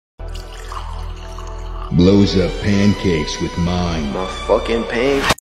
Blows up pancakes with mind | DreamSMP Animation
this audio has been all over my fyp and I just found every one funny, this audio just tickles me for some reason. So I wanted to make my own version, cuz it kind of feels like Wilbur and Tommy's dynamic

i also got to try out using green screen in premiere pro, so that was kind of fun

Hope you are having a good day :)